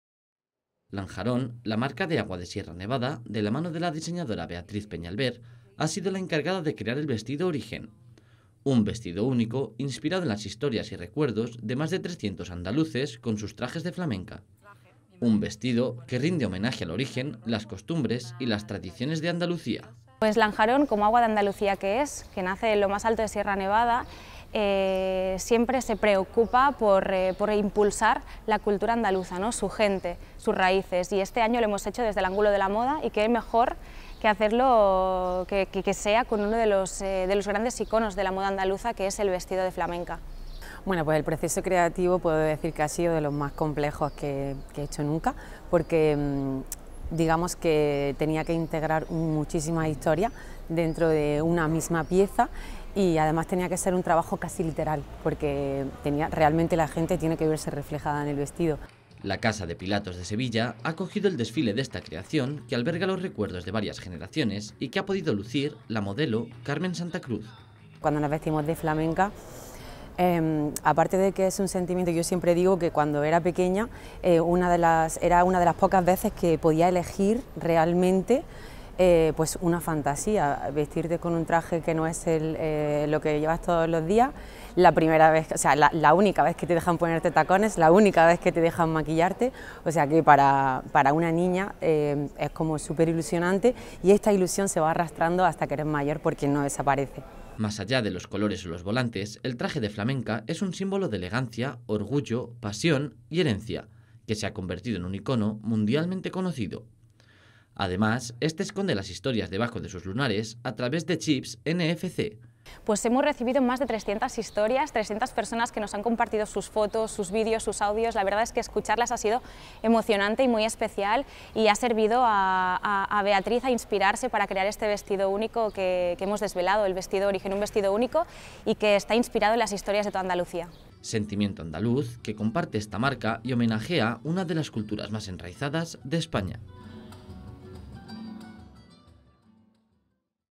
Más de 300 historias inspiran a Beatriz Peñalver para crear su nuevo vestido: ORIGEN
Lanjarón, la marca de agua de Sierra Nevada, de la mano de la diseñadora Beatriz Peñalver, ha sido la encargada de crear el vestido ORIGEN. Un vestido único, inspirado en las historias y recuerdos de más de 300 andaluces con sus trajes de flamenca. Un vestido que rinde homenaje al origen, las costumbres y las tradiciones de Andalucía.
La Casa de Pilatos de Sevilla ha acogido el desfile de esta creación que alberga los recuerdos de varias generaciones y que ha podido lucir la modelo Carmen Santa Cruz.
Más allá de los colores o los volantes, el traje de flamenca es un símbolo de elegancia, orgullo, pasión y herencia que se ha convertido en un icono mundialmente conocido. Además, este esconde las historias debajo de sus lunares a través de chips NFC, una tecnología nunca usada en moda.
Sentimiento andaluz que comparte esta marca y homenajea una de las culturas más enraizadas de España.